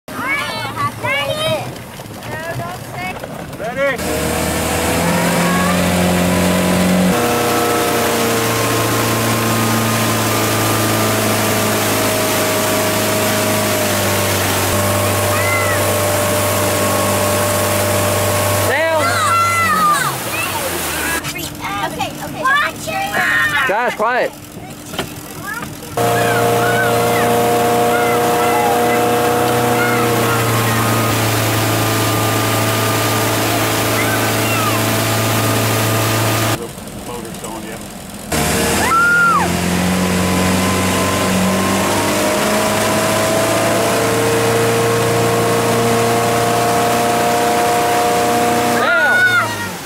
waterskiing on Backriver
Molly, Christa, and Joshua waterskiing on Backriver, July21, 2013